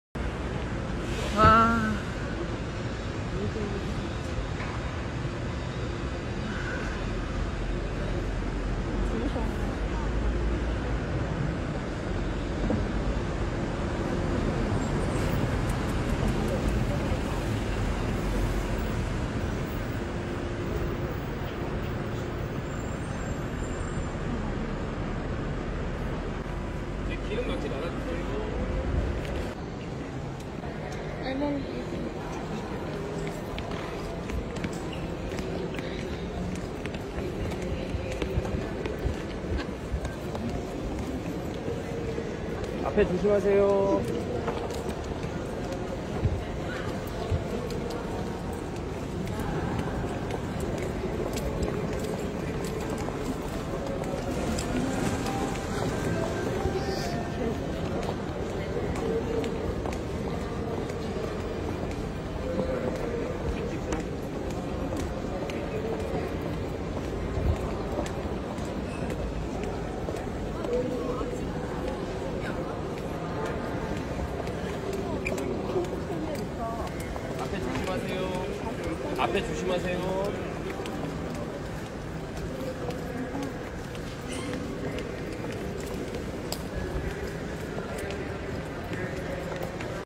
김현중 KIM HYUN JOONG RISING IMPACT WORLD TOUR fancam 2023.10.28 ✨️
Nuestro sol en el aeropuerto internacional rumbo a Chile 🇨🇱!!! 👏🏻👏🏻👏🏻🎉🎉🎉🎉🎉🥰🥰🥰😍😍mi corazón ya late a mil !! Te estamos esperando con ansias! ❤️❤️
Credit. khjsupporter
khjsupporter

jeanyerko

20231028

jdream_kyujong
 kimkyujong_art_official
hyunjoong860606 parkjungminsta youngsaeng17 hyungjunking kimhyunjoong 허영생 superstar heoyoungsaeng parkjungmin kimhyungjun international_thankyu_fanclub Singer 김현중 박정민 ~